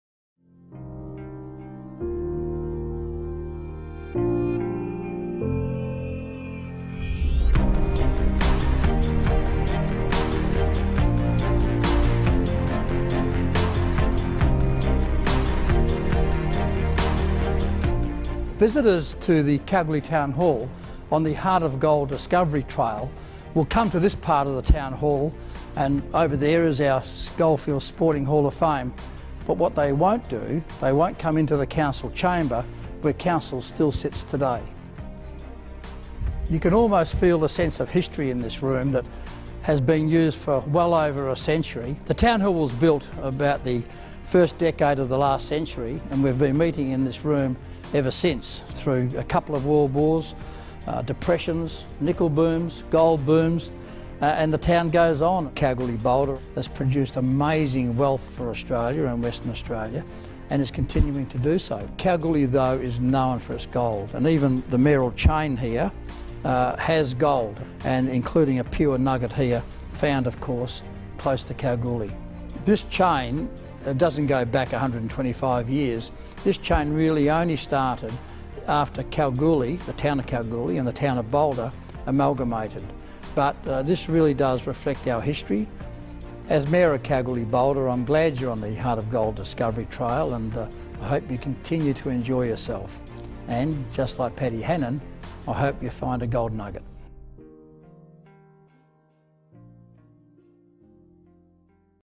Kalgoorlie heartofgold Discovery Trail - Mayor
Take a sneak peek at what you will unearth on Kalgoorlie’s heartofgold Discovery Trail. Kalgoorlie-Boulder Mayor John Bowler shows us around the Kalgoorlie Town Hall, including the council chamber, in this video at the third stop on the trail. You can even use our augmented reality game to find the mayoral gold chain inside the Kalgoorlie Town Hall. Download the free Heart of Gold Australia app to start your journey.